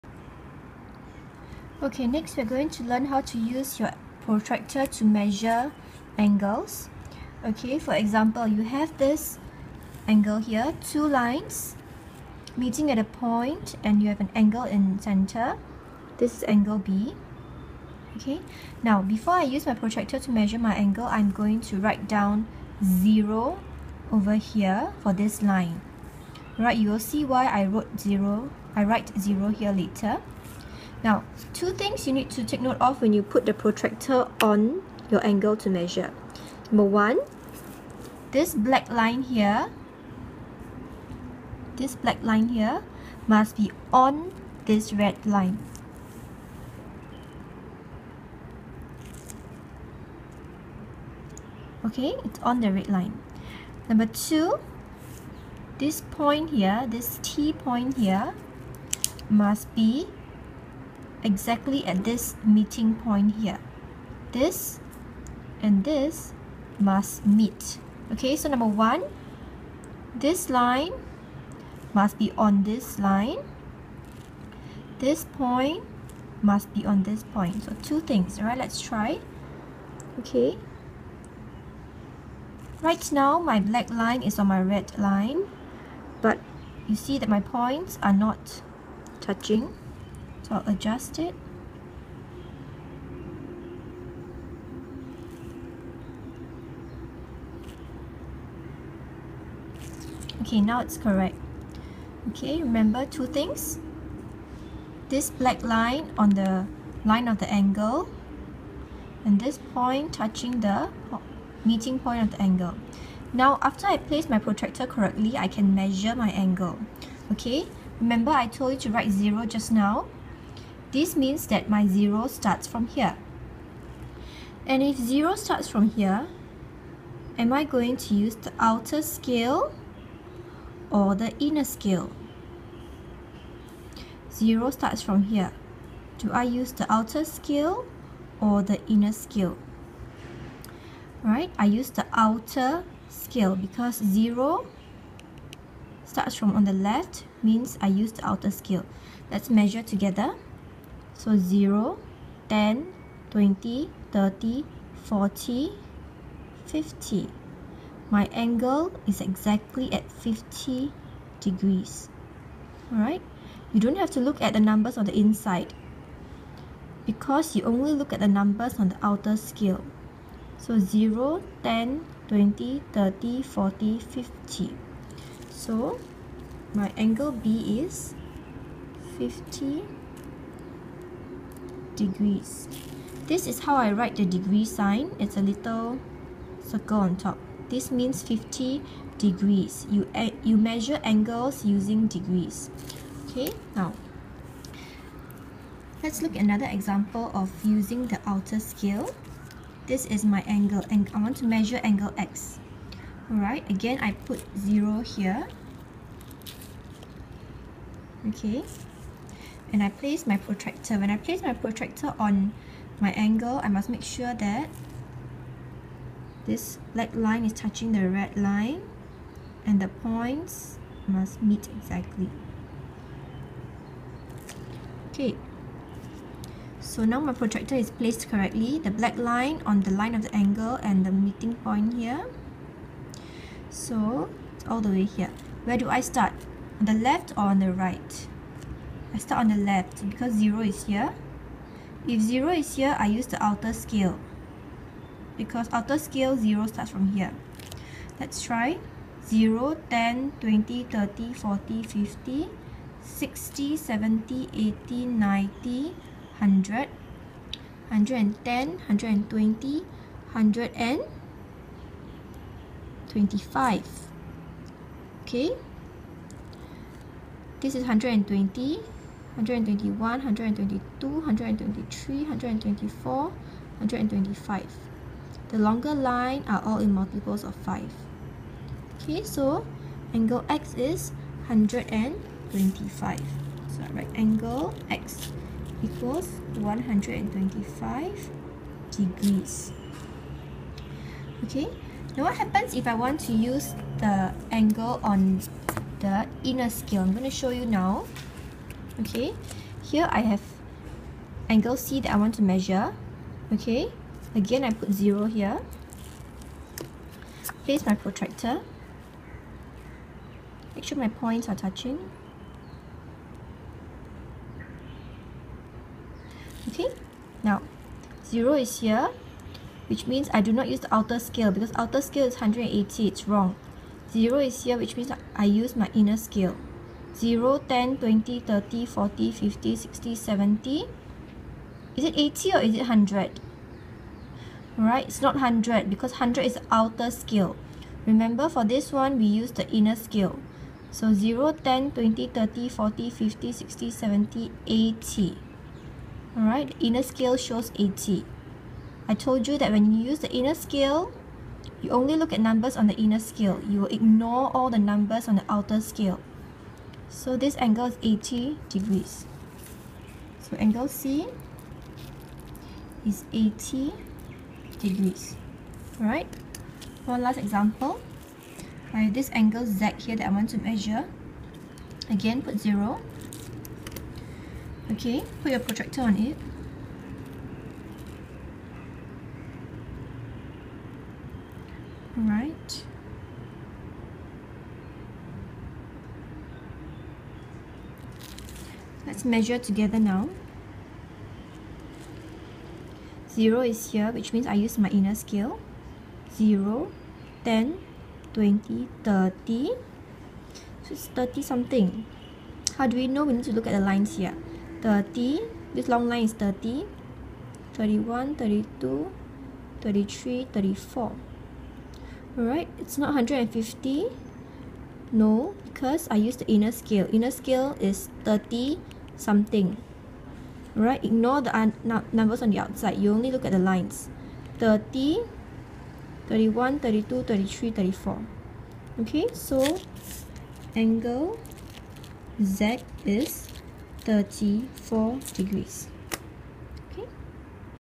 How to measure angles using a protractor (with super helpful tip!)
In this video, I teach you how to measure angles using a protractor (using outer scale & inner scale)